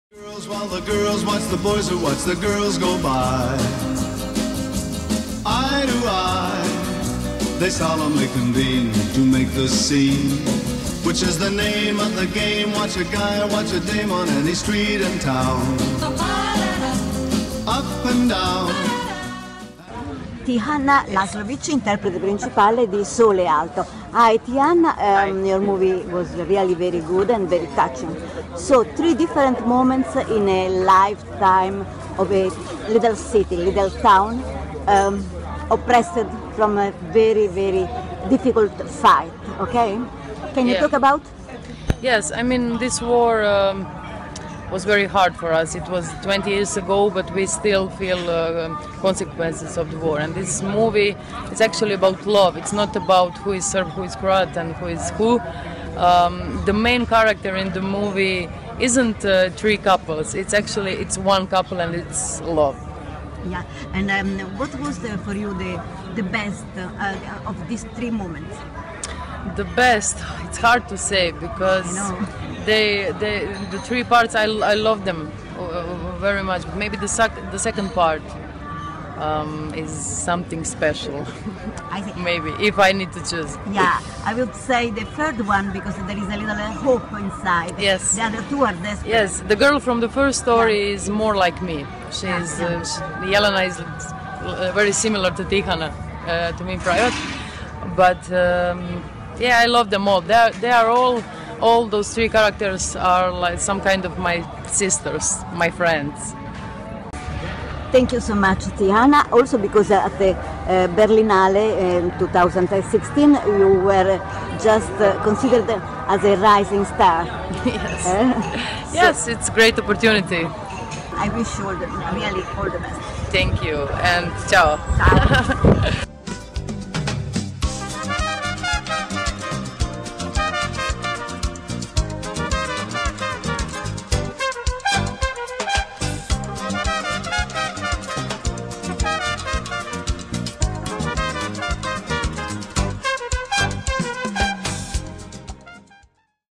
Tihana Lazovic protagonista di Sole alto premio Un certain regard a Cannes 2015
Reduce da Cannes 2015 da cui è uscito laureato con il premio della Giuria della prestigiosa sezione Un Certain Regard,  Sole Alto di Dalibor Matanic è un magnifico film per raccontarci, con l’amore,  la guerra, le incomprensioni razziali e l’odio insanabile di due popoli vicini e pur così intollerantemente lontani. I serbi e i croati.
Servizio e riprese di Mariangiola Castrovilli per VisumTV